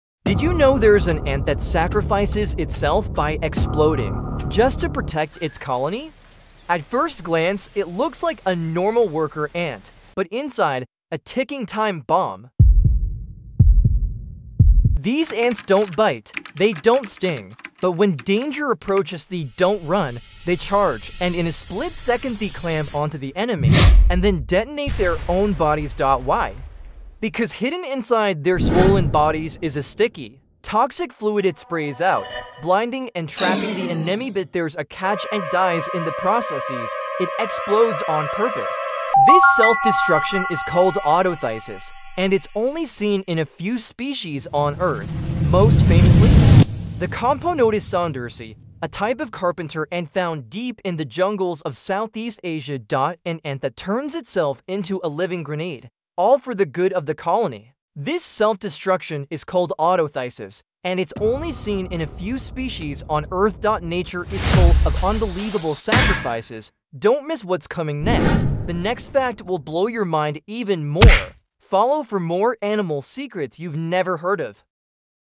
Which Animal EXPLODES Like a Bomb?”
There's an animal that self-destructs like a suicide bomber — not out of rage, but loyalty. Discover the insane defense of one of the world’s most extreme insects. Watch till the end to find out who the real "living bomb" is.